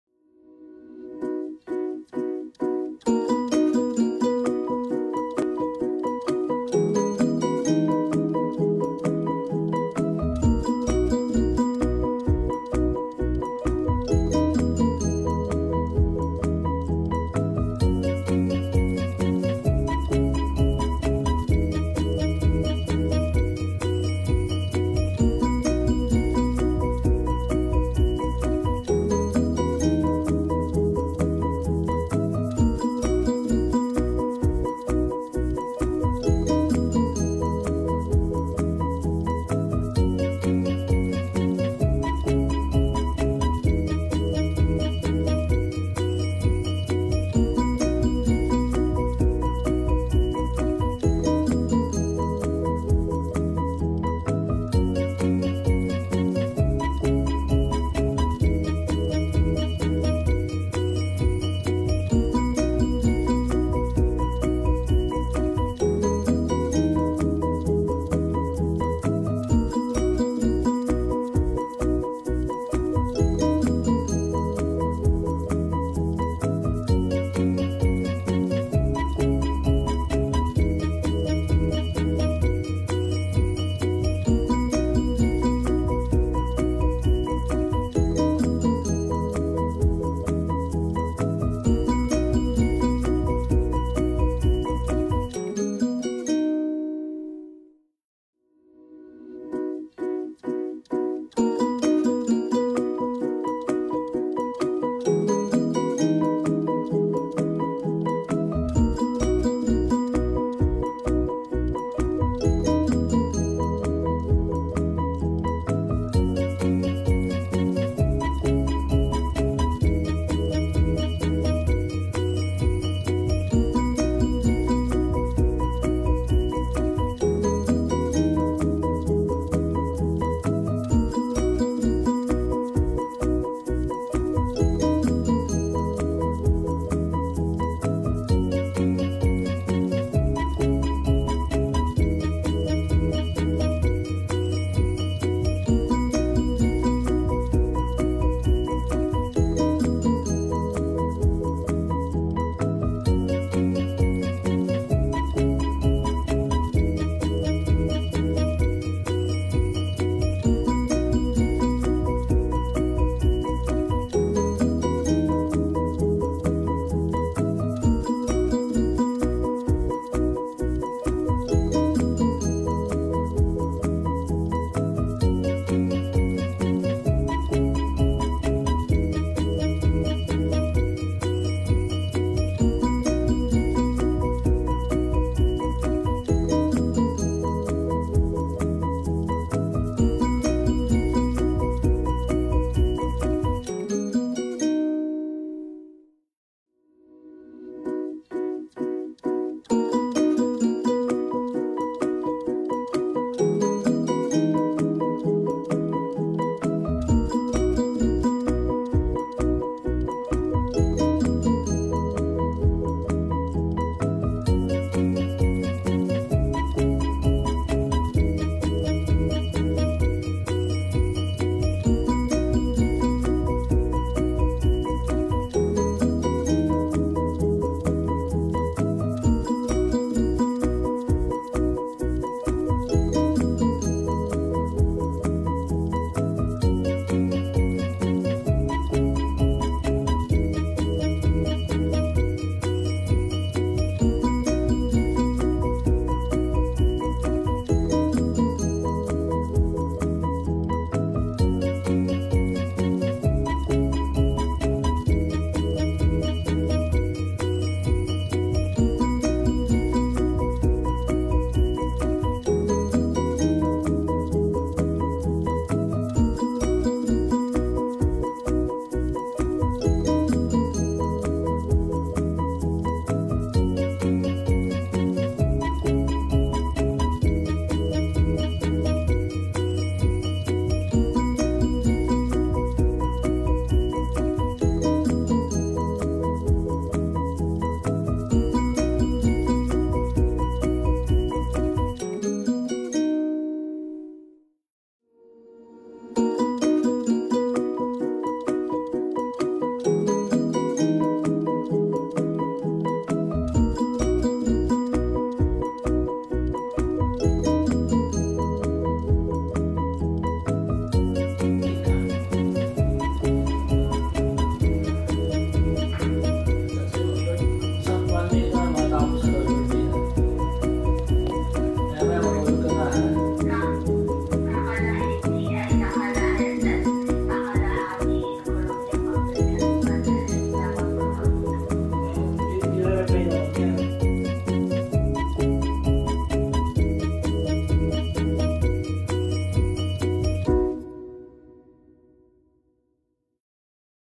Sinigang na Hipon
Sinigang na Hipon is a type of Filipino sour soup, wherein shrimp is used as the main ingredient. This dish also includes a variety of vegetables such as daikon radish, snake beans, okra, and eggplant. This dish is best served during cold weather. It is best enjoyed hot with a cup of white rice.